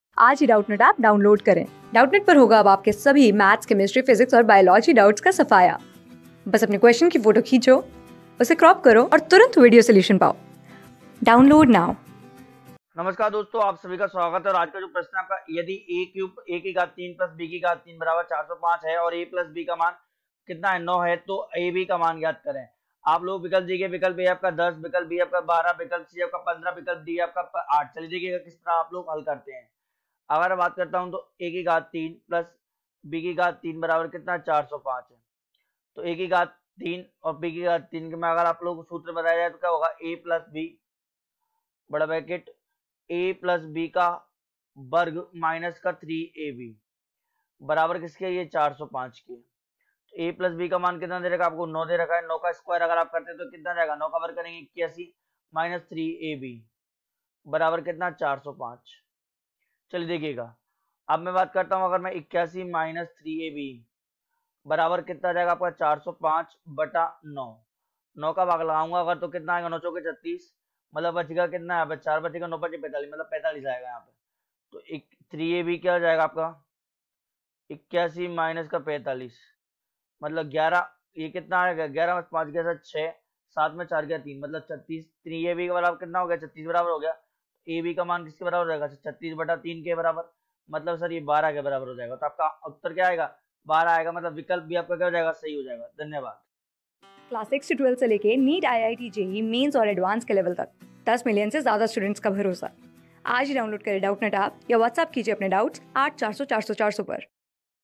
यदि a^(3) + b^(3) = 405 और a + b = 9 है, तो ab का मान ज्ञात करें। | CLASS 14 | COMBINED GRADUAT...
यदि a^(3) + b^(3) = 405 और a + b = 9 है, तो ab का मान ज्ञात करें।
Class: 14
Subject: MATHS
Chapter: COMBINED GRADUATE LEVEL EXAMINATION 2020 TIER 1
Board:GOVT

👉 Have Any Query? Ask Us.